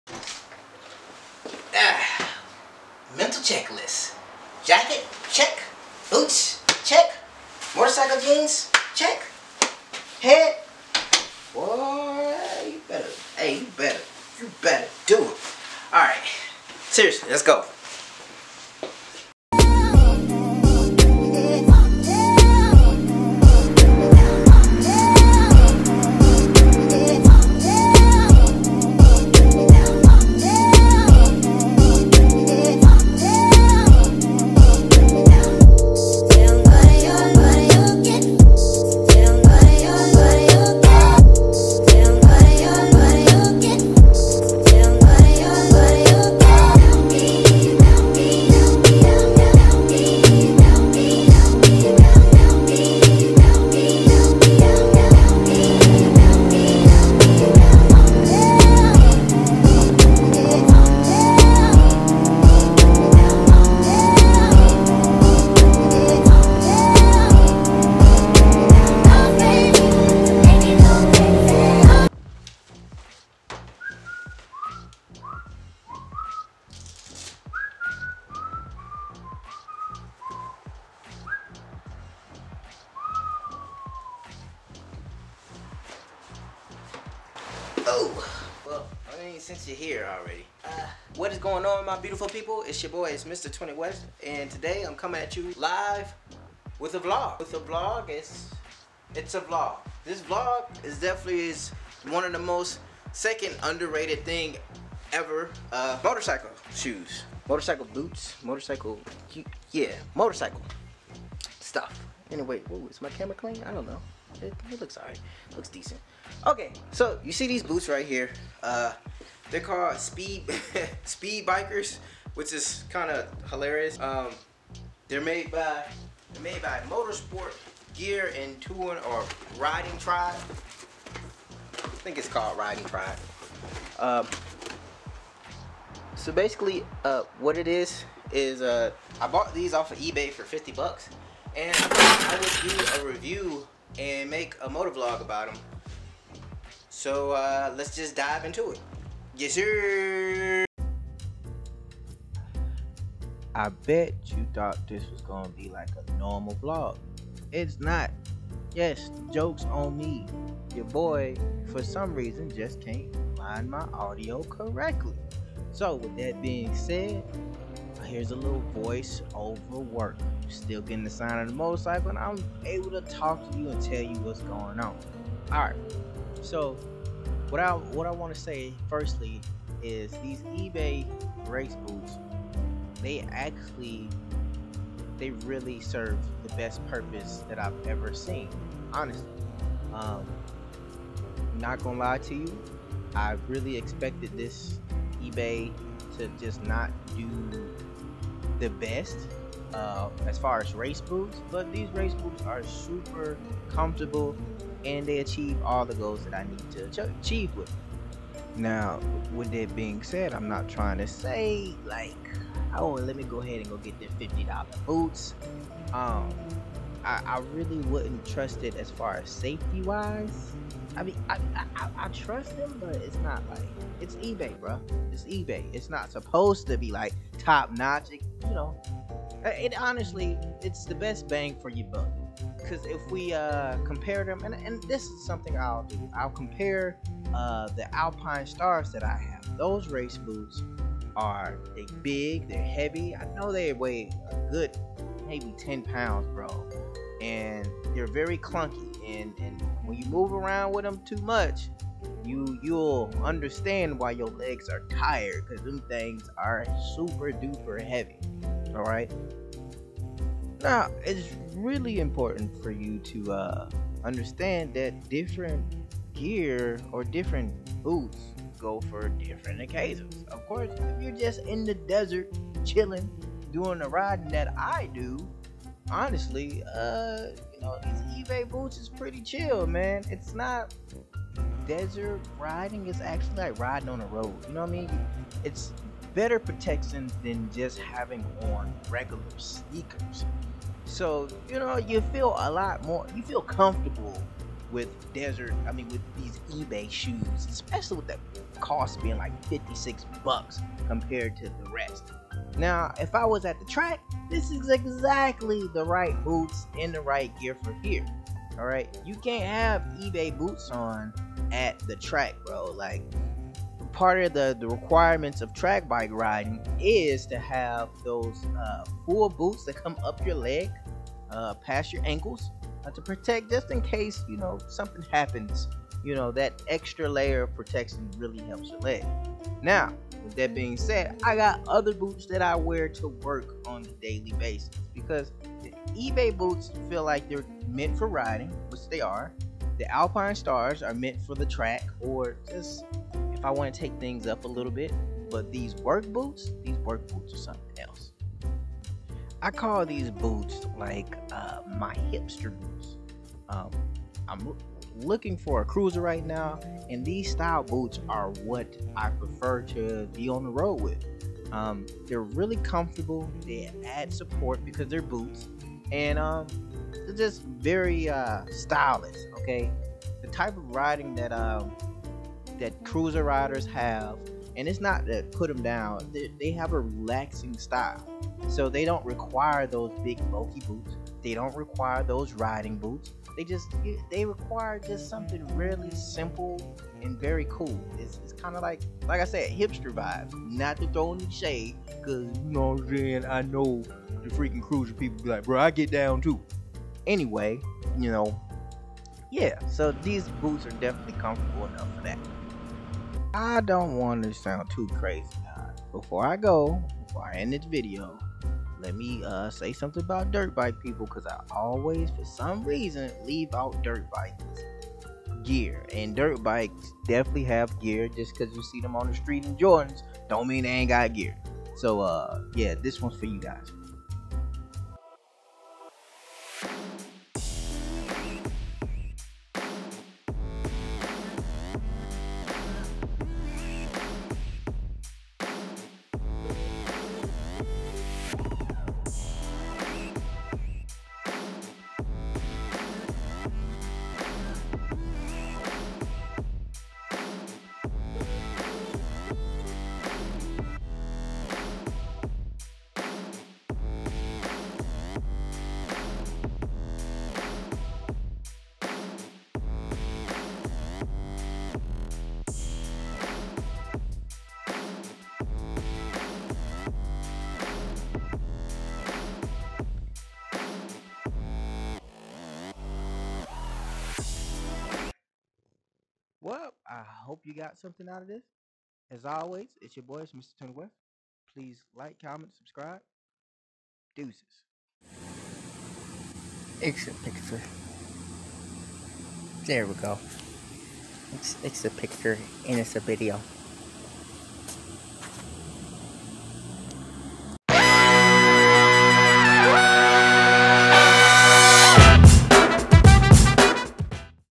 My super cheap Ebay Motorcycle Boots (Review )By mr20west
Hello In the market for some dope Motorcycle gear that's not only cheap but good. Here's my ebay boots review .

Instgram :Mr20.west

Before you start to look down on gear make sure you know that riding with out it can be a matter of life and death.